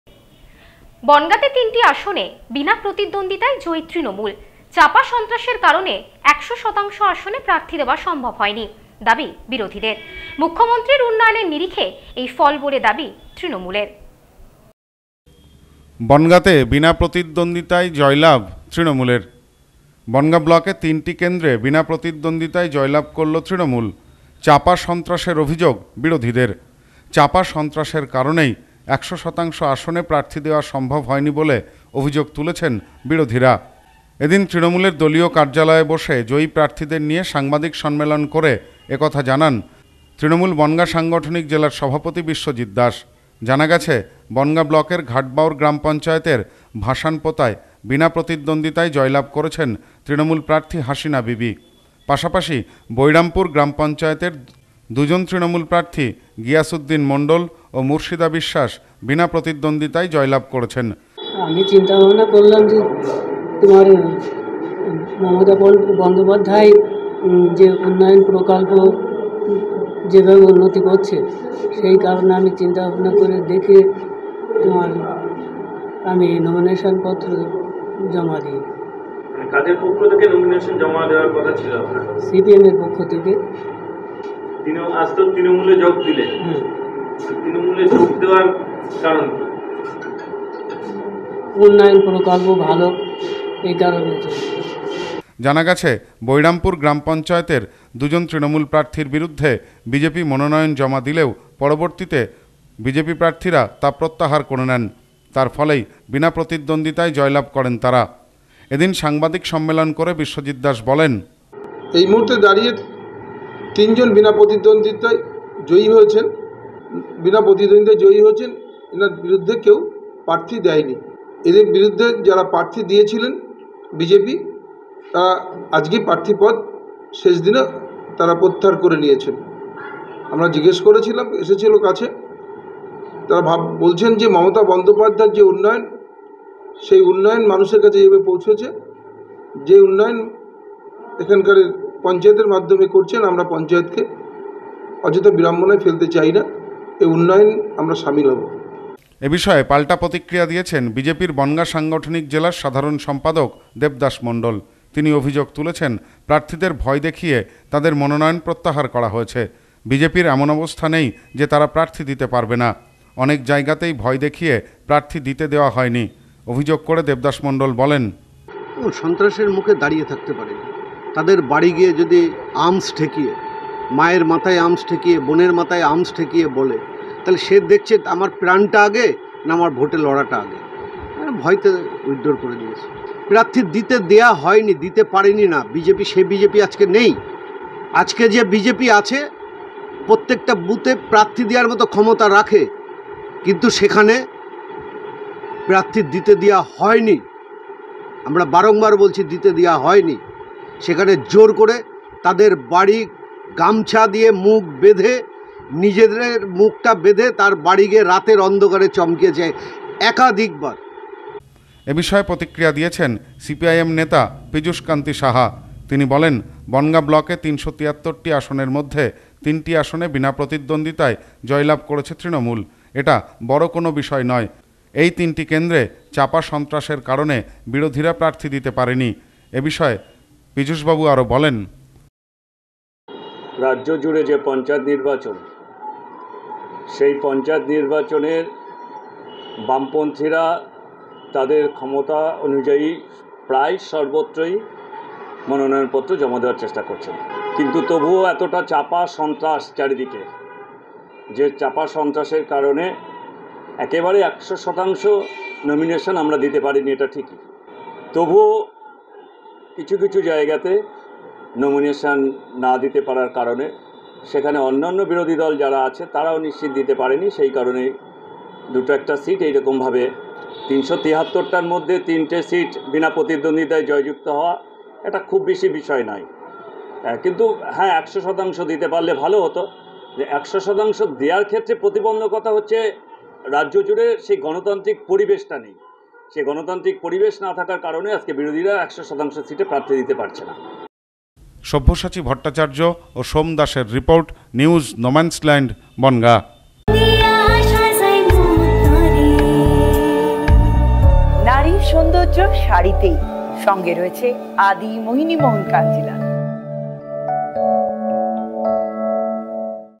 বনগাঁতে তিনটি আসনে বিনা প্রতিদ্বন্দ্বিতায় জয়ী তৃণমূল।
বনগাঁতে তিনটি আসনে বিনা প্রতিদ্বন্দ্বিতায় জয়ী তৃণমূল। চাপা সন্ত্রাসের কারণে ১০০ শতাংশ আসনে প্রার্থী দেওয়া সম্ভব হয়নি দাবী বিরোধীদের। মুখ্যমন্ত্রীর উন্নয়নে নিরিখেই এই ফল বলে দাবী তৃণমূলের।